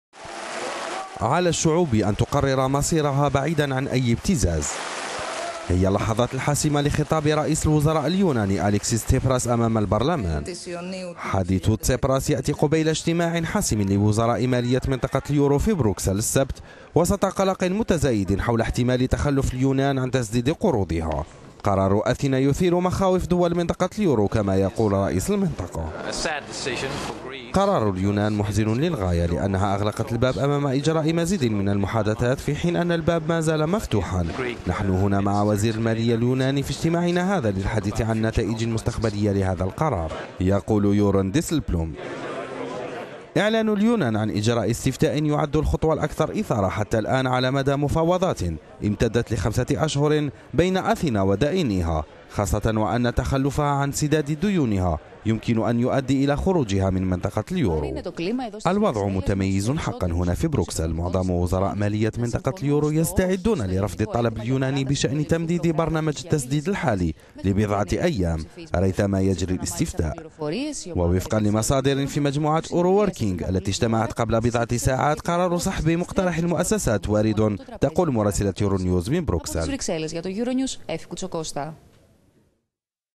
اليونان يحدث زلزالا في منطقة اليورو قبيل إجتماع وزارء الخارجية في بروكسل
على الشعوب أن تقرر مصيرها بعيدًا عن أي ابتزاز، والاستفتاء سيجرى في الخامس من جويلية/ يوليو المقبل، هي اللحظات الحاسمة لخطاب رئيس الوزراء اليوناني أليكسيس تسيبراس أمام البرلمان.
حديث تسيبراس يأتي قبيل اجتماع حاسم لوزراء مالية منطقة اليورو في بروكسل، السبت، وسط قلق متزايد حول احتمال تخلف اليونان،عن تسديد قروضها.
قرار اليونان يثير مخاوف دول  منطقة اليورو، كما يقول رئيس منطقة اليورو يورن ديسلبلوم :

" قرار اليونان محزن للغاية لأنها أغلقت الباب أمام إجراء مزيد من المحادثات في حين أن الباب ما زال مفتو…